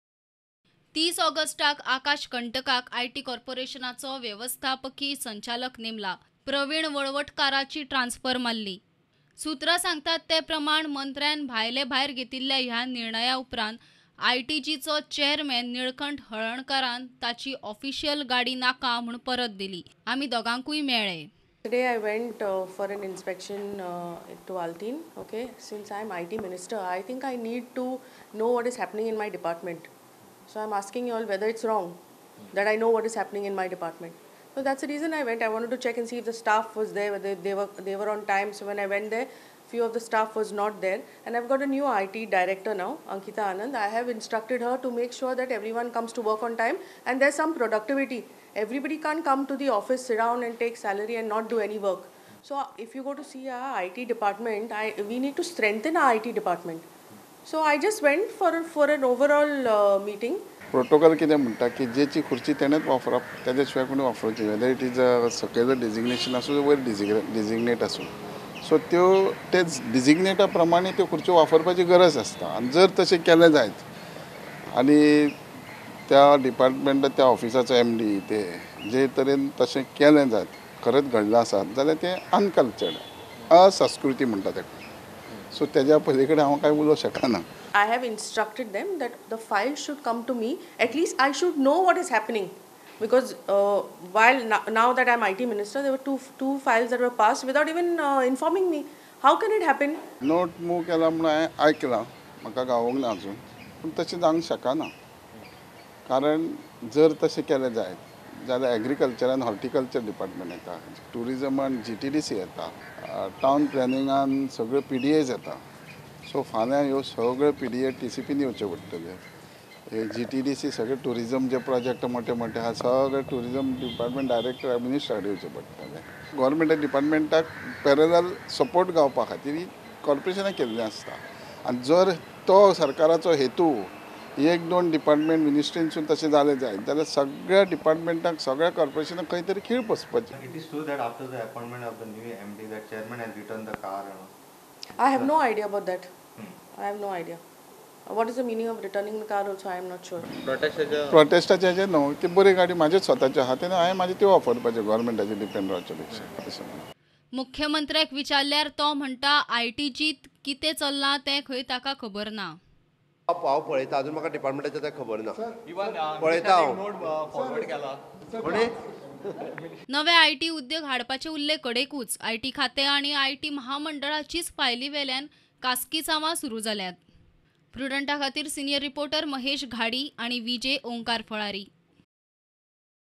JENNIFER - NILKANTH SPAR OVER CONTROL OF CORPORATION; CM TO INTERVENE_Prudent Media Goa
All is not well in the Information Technology Department of the State Government. Minister Jennifer Monserrate has issued a note to Managing Director of Info Tech Corporation of Goa Limited (ITG), stating that all files pertaining to ITG should be moved through her only. Chairman of the ITG Nilkant Harlankar has stated that he was not taken into confidence by the IT Minister before issuing such kind of notice. Meanwhile, Chief Minister Pramod Sawant has stated that he has no idea about the IT Department CM